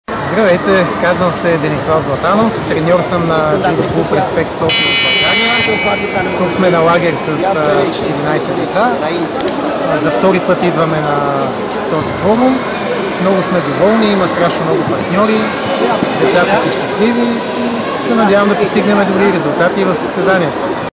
Judo TrainingCamp Izmit Turkey 2023 Denislav Zlatanov (Bulgaria)
Training camp of Olympic champion Hüseyin Ozkan in Turkey.
Video by Roman Karasev ROKA JUDO